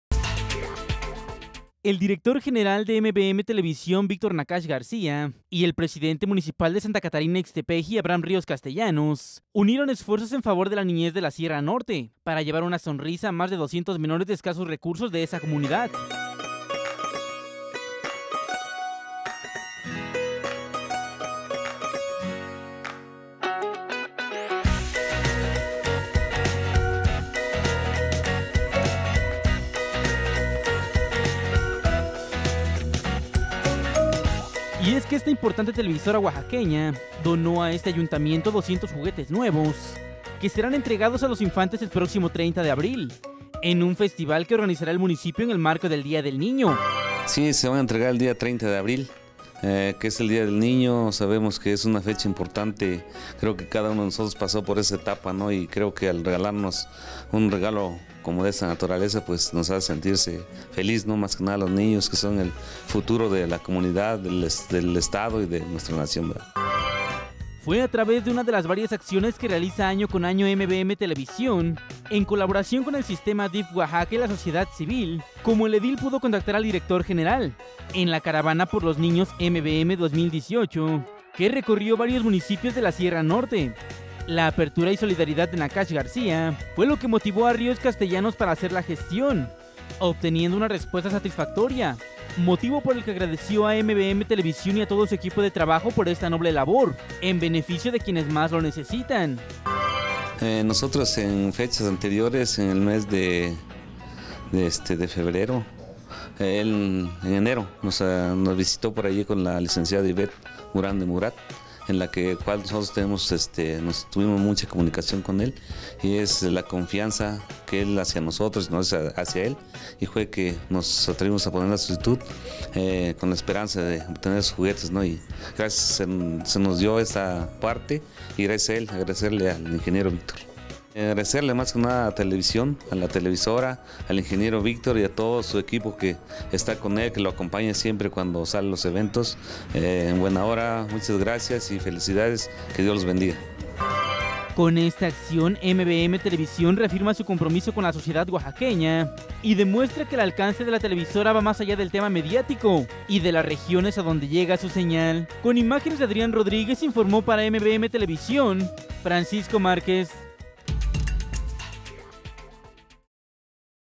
Entrega de juguetes para niños de Santa Catarina Ixtepeji
MVMTelevisión, a través de su Director General Víctor Nakash García, hacen posible la sonrisa de cientos de niños de la Sierra Norte.